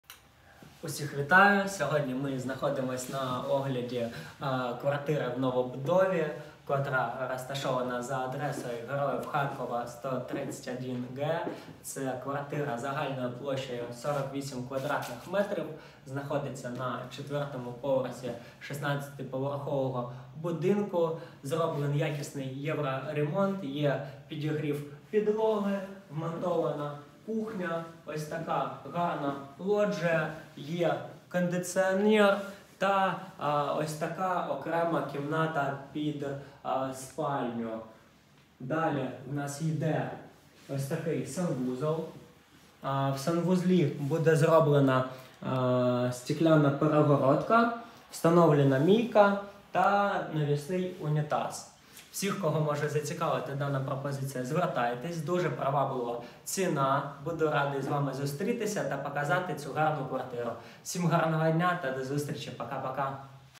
#9501 Продаж Квартири у ЖК Іскринський. Пр-т Героїв Харкова
Продається квартира у ЖК Іскринський!

Адреса: проспект Перемоги 131Г, 16-поверховий будинок, поруч з метро "Захистники України".

Загальна площа: 48 м²

Особливості:

Укомплектована всіма меблями та технікою
Підігрів підлоги
Вбудований холодильник
Індукційна плита
Кондиціонер
Двоспальне ліжко
Встановлені виходи під інтернет, телевізор, домофон
Лоджія
Ванна кімната:

Гігієнічний душ
Навісний унітаз
Раковина
Додаткові переваги: Просторий та світлий інтер'єр, зручне розташування, близькість до транспорту та інфраструктури.

Не втрачайте можливість стати власником комфортної квартири в зручному районі!

єБаза нерухомості бот-База недвижимости Харьков
Твій персональний помічник на ринку нерухомості 🏡
🏬 оренда
🏦 купівля-продаж
🏠 житлова та комерційна нерухомість